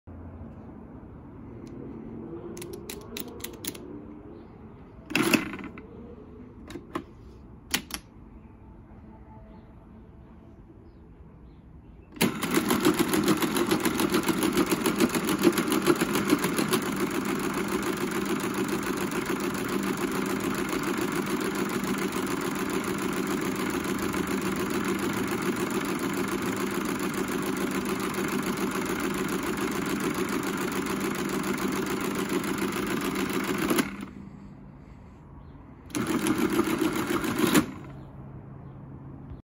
What happens when you divide by zero on a mechanical calculator?
This is how the Friden STW-10 handles 12345 ÷ 0.

This calculator divides by trying to repeatedly subtract so it gets stuck in an infinite loop trying to subtract zero from the dividend. Fortunately the designers included  a “DIV STOP” lever which breaks the cycle.